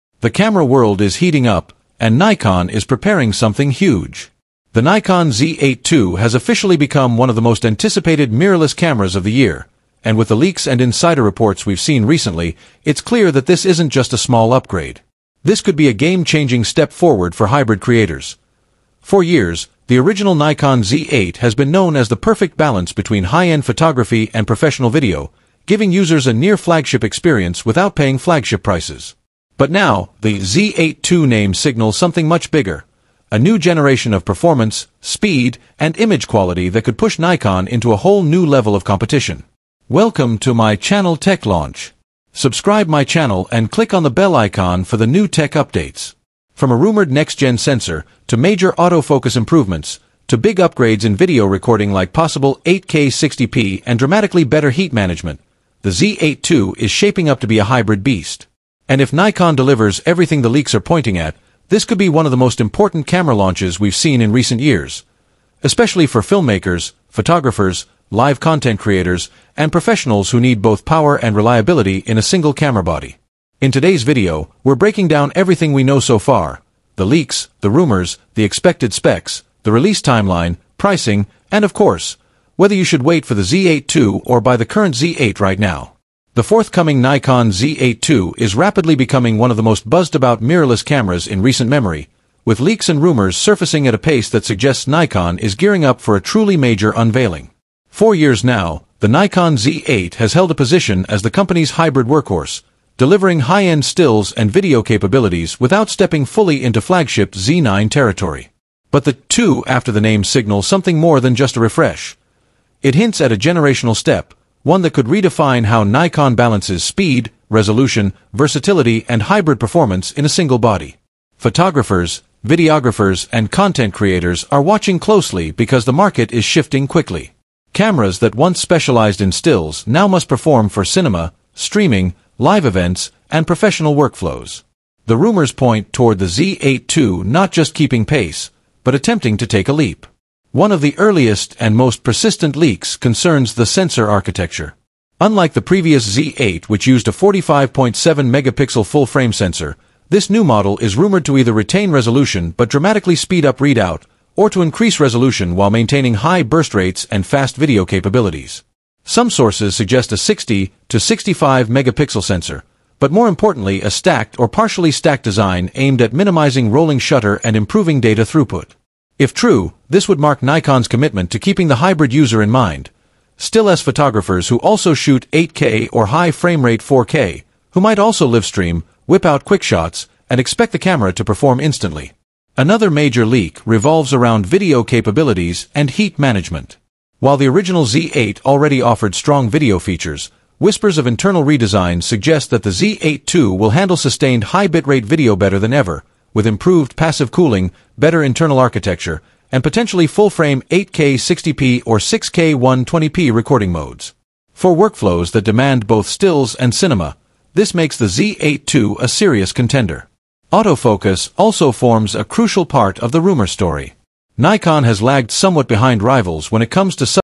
Why the Nikon Z8 II is the Best Mirrorless Camera Right Now | 45.7MP, 8K & More
Nikon Z8 II full hands-on review (2025) — humne low light, autofocus, 8K video, battery aur real-world image/video samples test kiye. Kya Z8 II upgrade worth hai agar aap already Z8 owner ho? Short verdict + recommended settings inside.